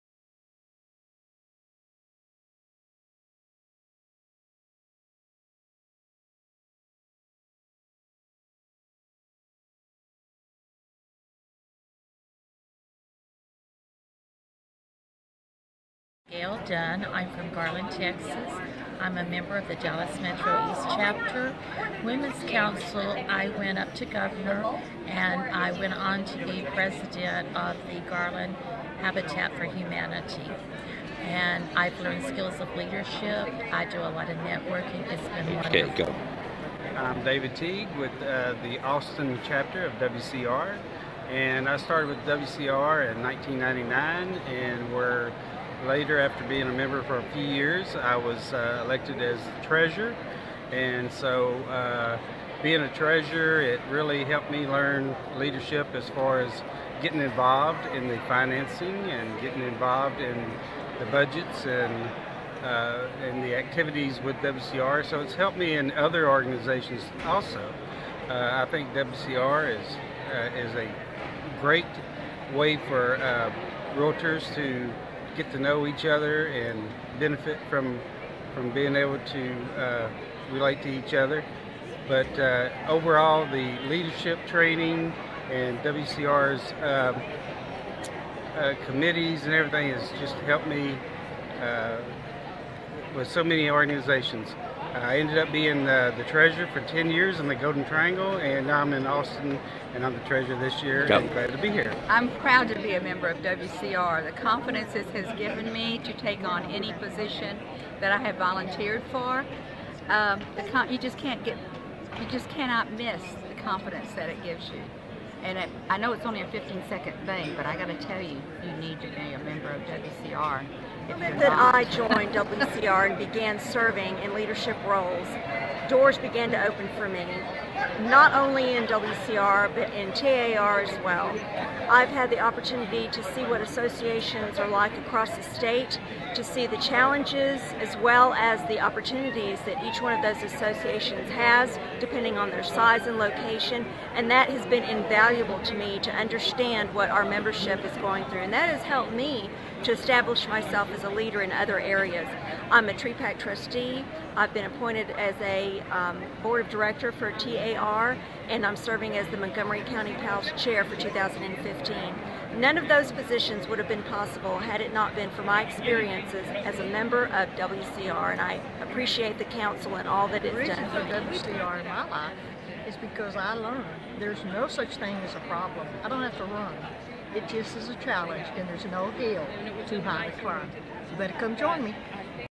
How has WCR Membership led to Leadership Opportunities for YOU?
This video is intended to supplement the WCR Texas Chapter brochure entitled "Pathway to Leadership".